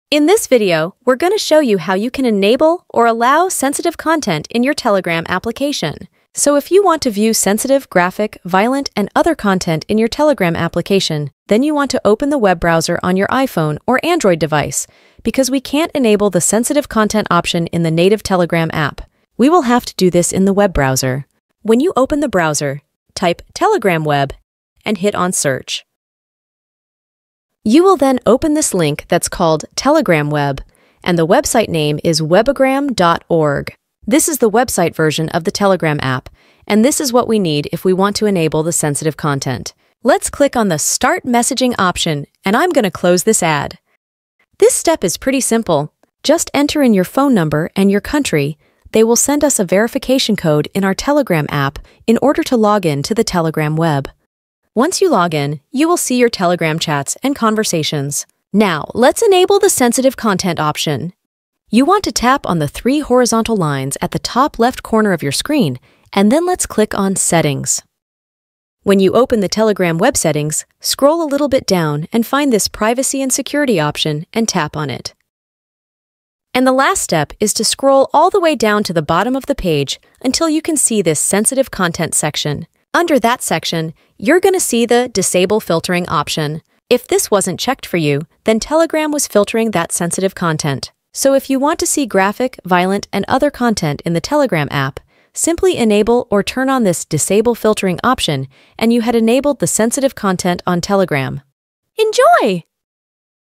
ENABLE Sensitive Content on Telegram (2025) [iOS & Android]
In this video, we guide you through the process of enabling sensitive content on Telegram for both iOS and Android devices.